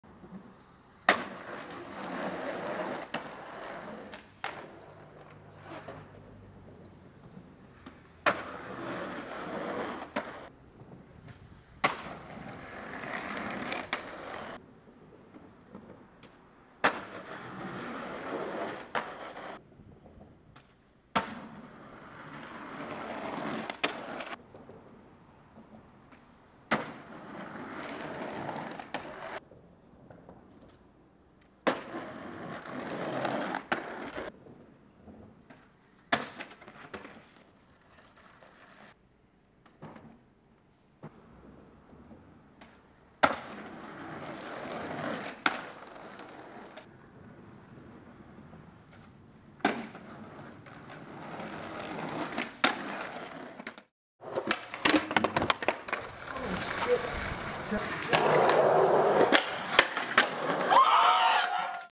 Spot Check In #1: Roof to Bank SF
Instagrams: @samgpics, @freakwencyskateboards

Here is a new segment I am starting called Spot Check In! Where we go to a well known spot and do some tricks on it!